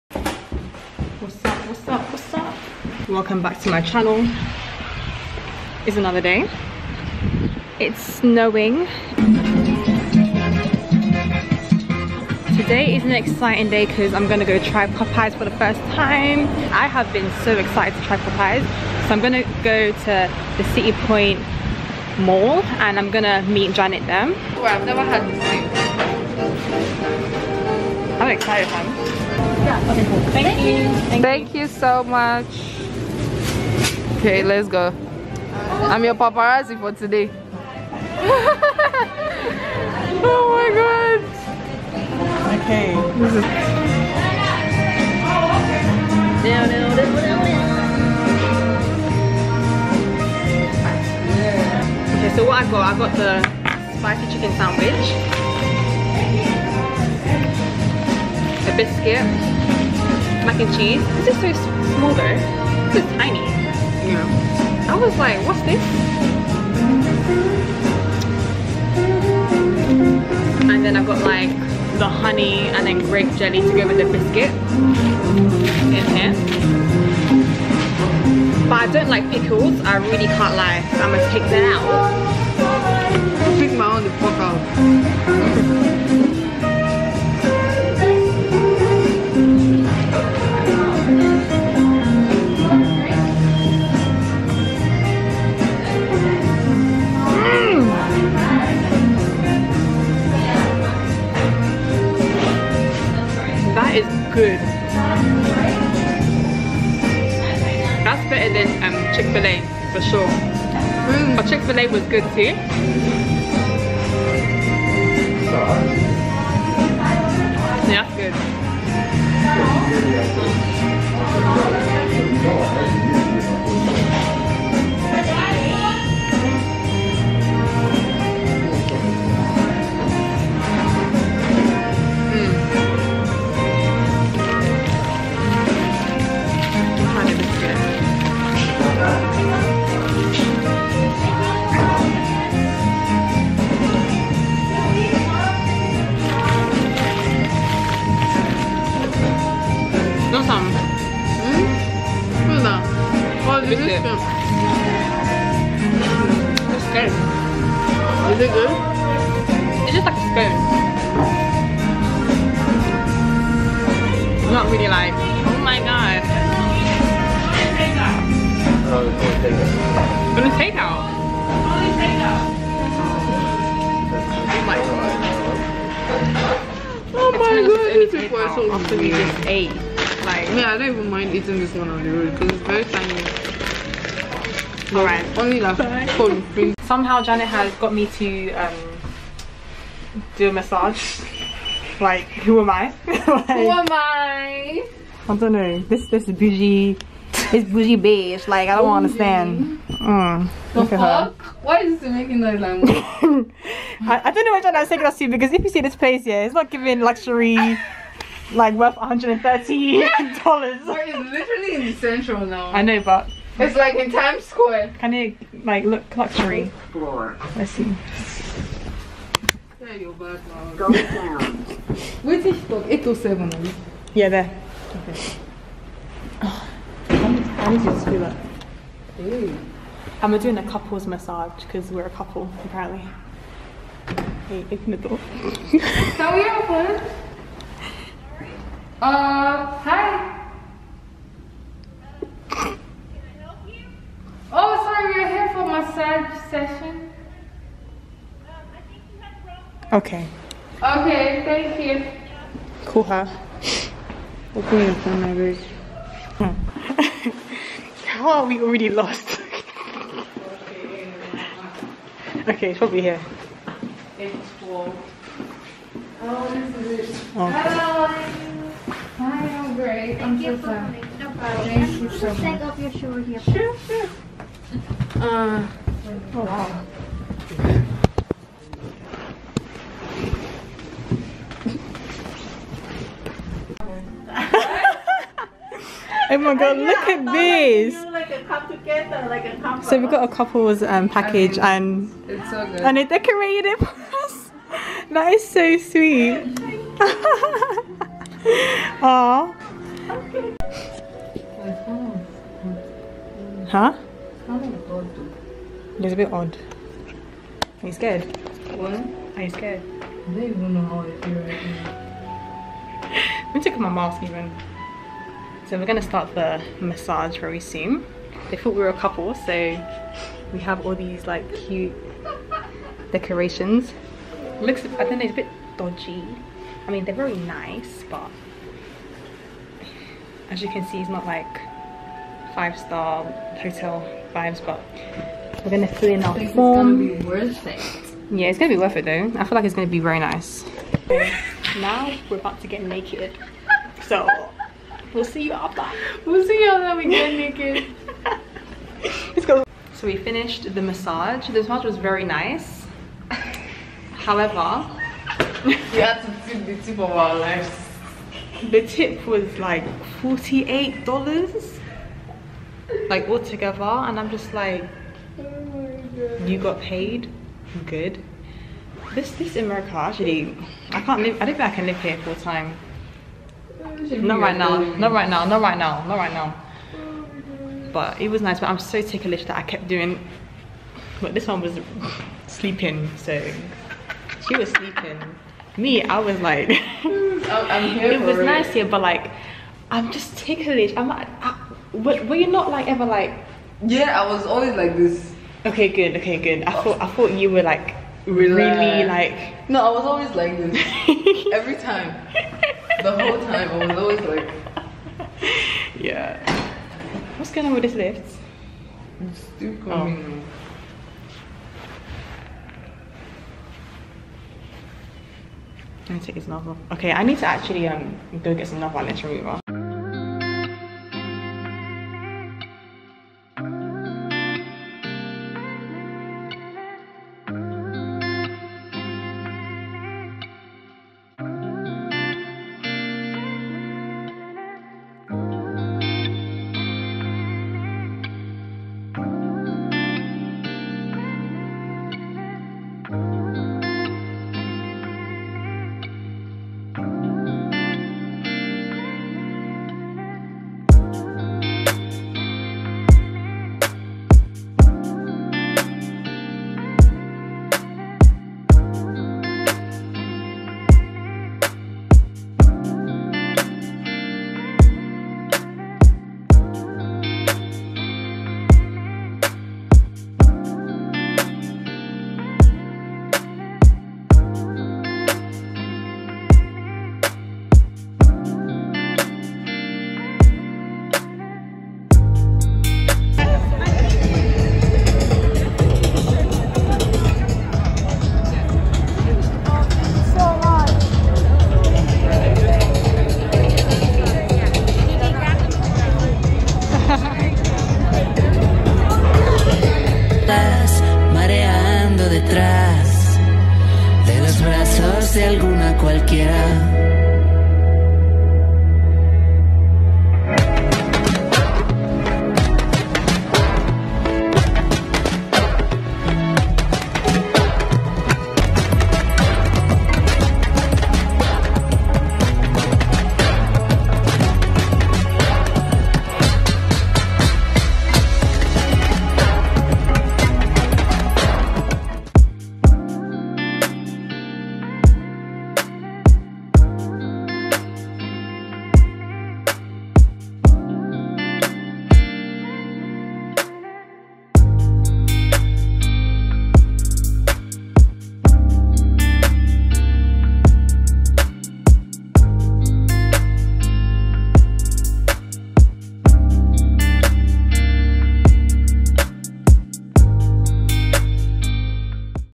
NYFW "Believe" ep 7 - Gabriela hearst show + British trying Popeyes for the first time + more
My motto for life, let's be on this journey together.